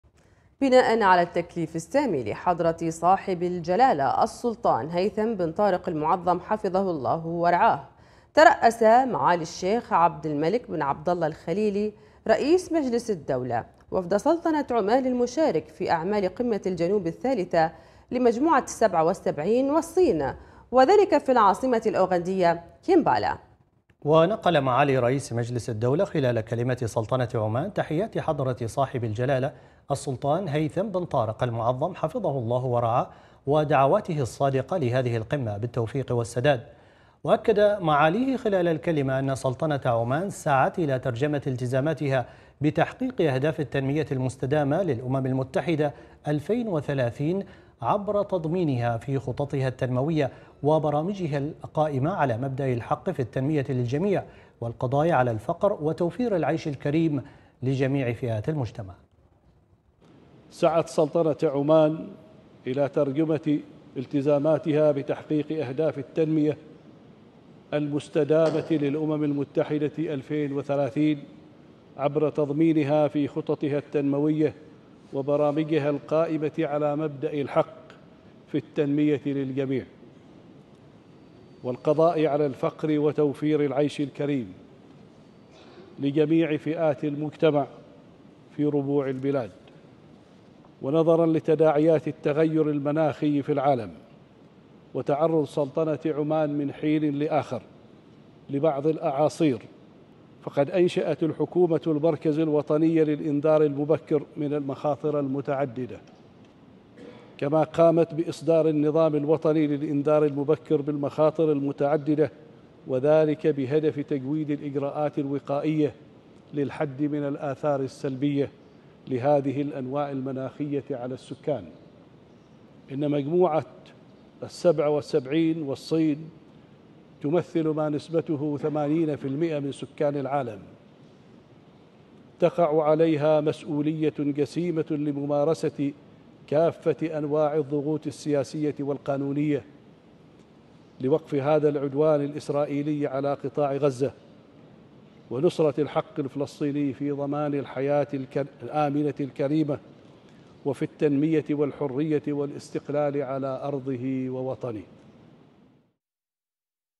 بتكليف سام .. رئيس مجلسِ الدولة يترأس وفد سلطنة عمان في أعمال قمة "الجنوب الثالثة لمجموعة الـ 77
بتكليف سام .. رئيس مجلسِ الدولة يترأس وفد سلطنة عمان في أعمال قمة "الجنوب الثالثة لمجموعة الـ 77 والصين" بالعاصمة الأوغندية كمبالا